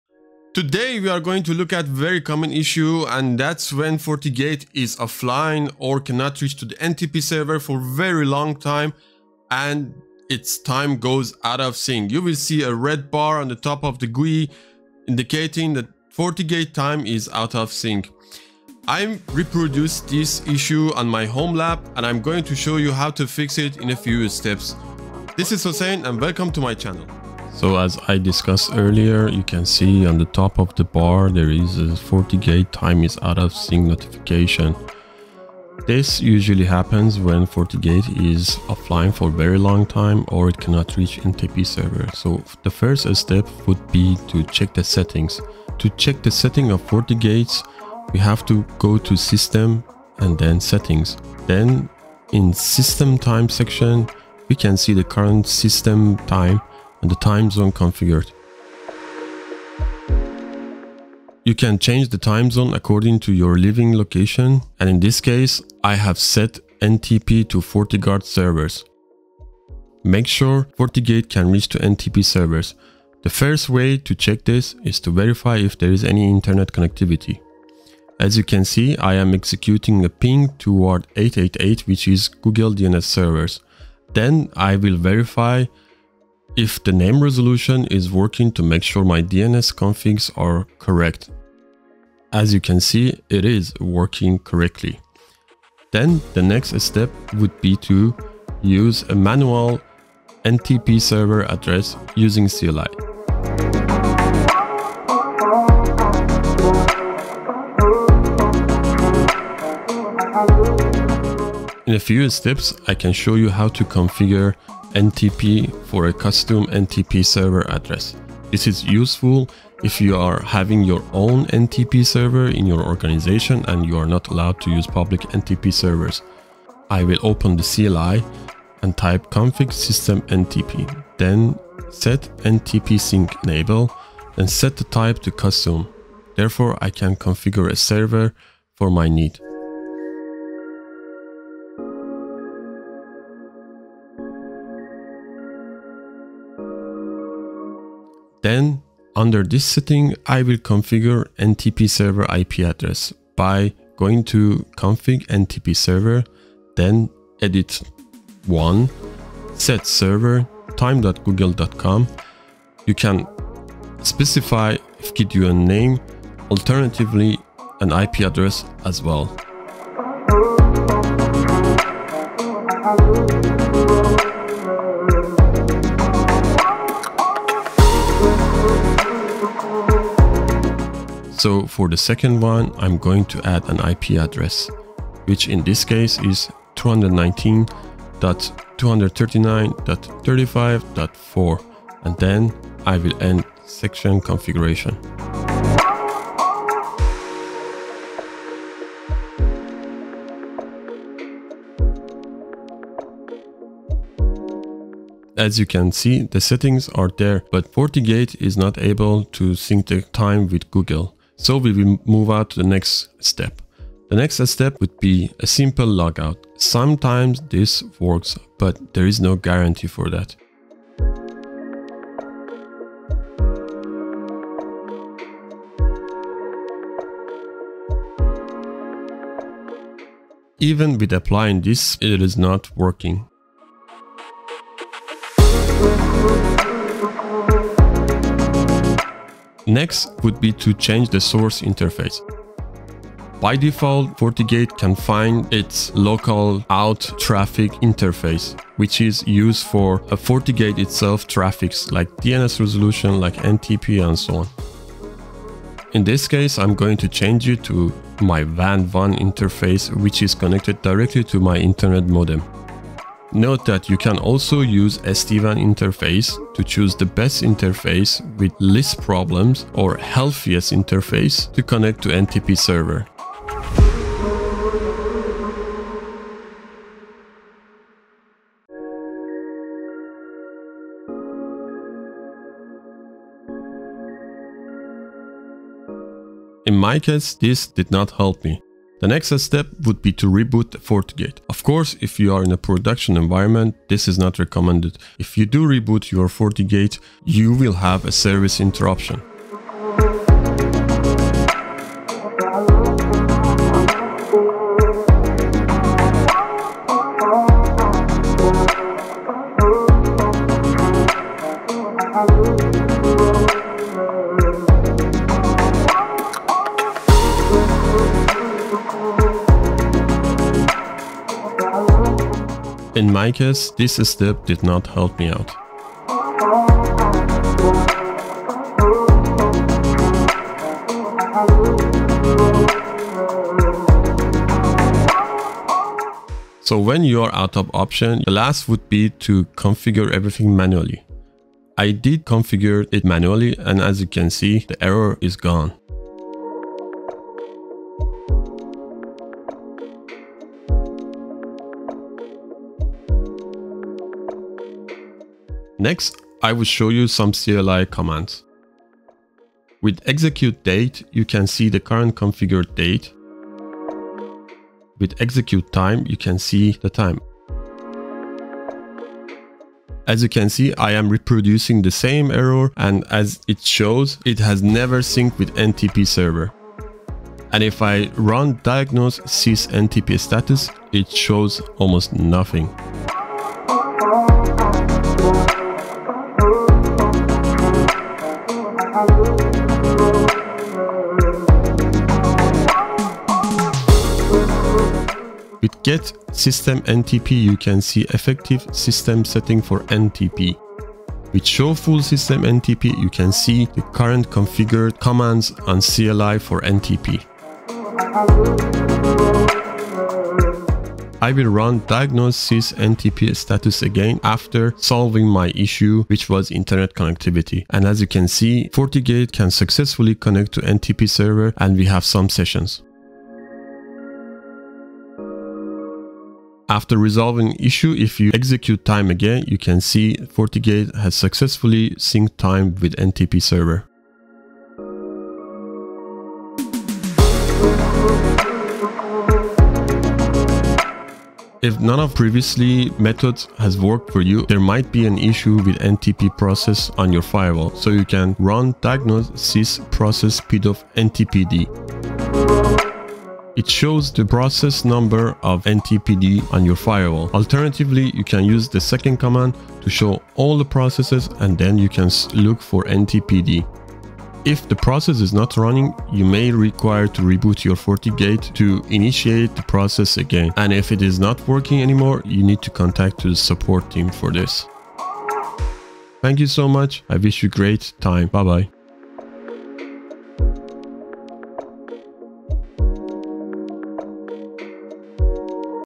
1- Troubleshooting Fortigate NTP - Fortigate time is out of sync
One of the issues I usually face is the Fortigate time goes out of sync. In this video, I'm going to show you how to fix this issue with very simple steps.

00:00 Intro
00:30 Check settings
01:14 Verify the internet connectivity
01:27 Verify name resolution
01:41 Manually config a server
03:30 Logout
04:01 Change the source interface
05:00 Reboot
05:35 Manually configure
05:59 Check date CLI
06:12 Check time CLI
06:19 Check NTP status
06:33 Check effective NTP settings
06:42 Check NTP configuration
06:57 Check NTP status
07:24 Check NTP process
07:55 End